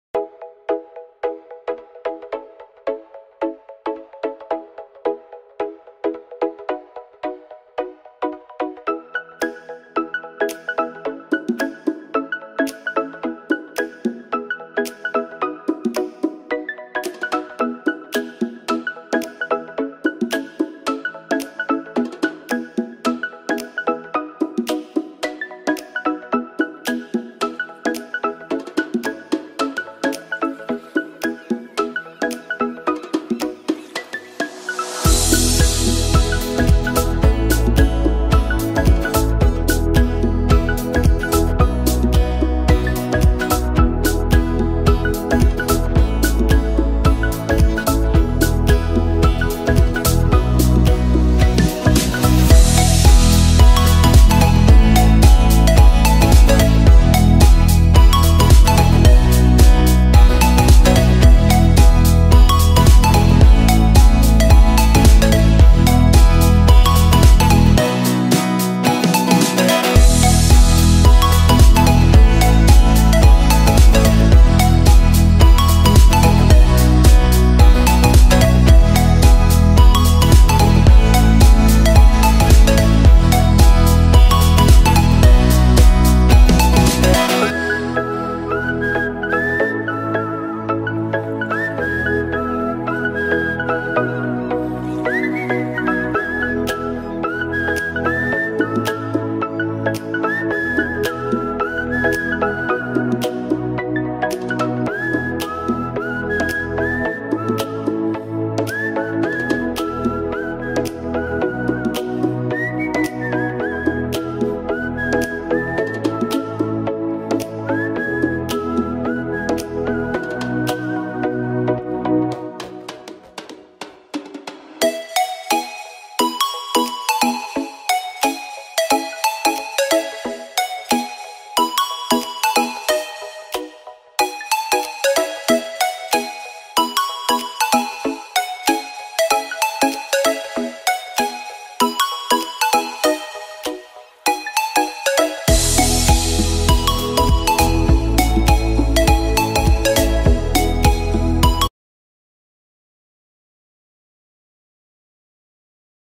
How To Assamble Mika Micky Bedside Sleeper Easy Folding Portable Crib?
Installation of the Mika Micky Bedside Sleeper Easy Folding Portable Crib is easy, Its tool frss assembly and hardly take less than 3-4 min to assemble the Mika Micky Bedside Sleeper Easy Folding Portable Crib.